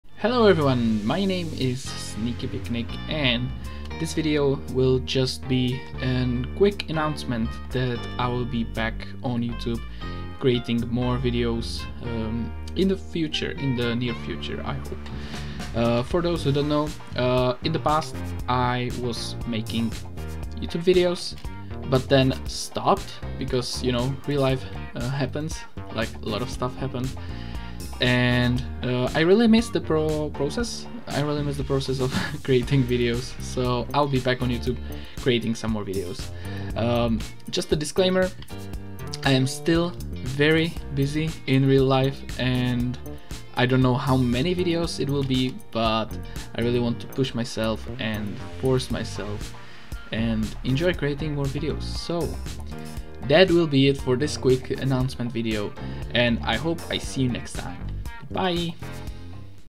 Jumping back on Youtube!!
I'm back on youtube to create more minecraft videos!
I really missed creating videos so I decided to come back.